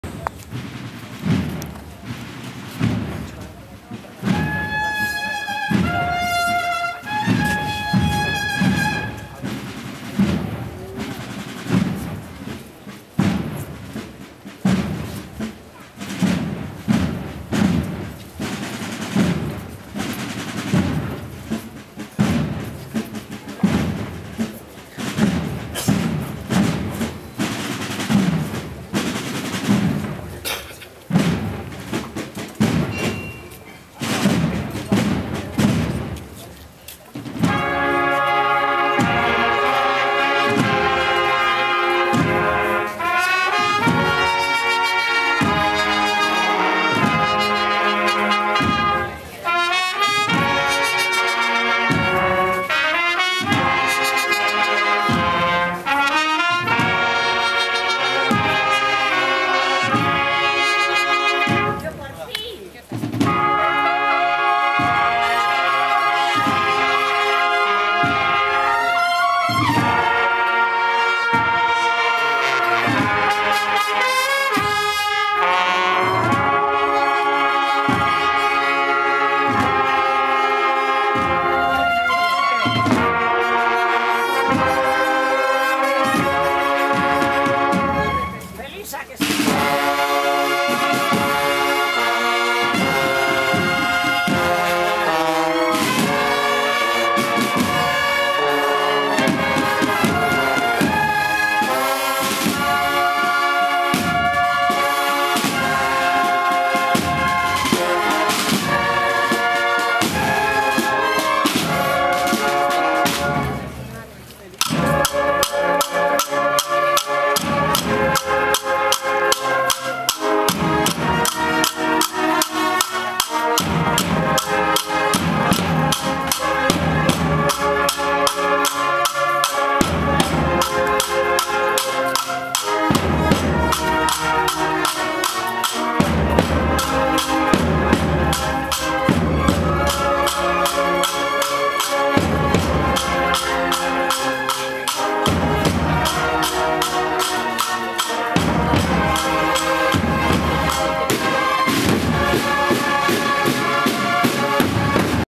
Procesión Via Matris en la Semana Santa de Ávila
La Semana Santa de Ávila da comienzo, el Viernes de Dolores, con la procesión del 'Via Matris' o Camino de la Madre, que parte del Convento de La Santa y recorre las principales calles del casco histórico. Una docena de reclusas del Centro Penitenciario de Brieva acompañan la imagen del Santísimo Cristo de los Afligidos y de Santa María Consuelo de los Afligidos, mientras el Cuarteto Zarabanda y la Banda de Cornetas y Tambores 'El Amarrado' ponen la nota musical al desfile, organizado por la Cofradía del Santísimo Cristo de los Afligidos, vestidos sus miembros con el hábito del Carmelo y capucha y cíngulo beige.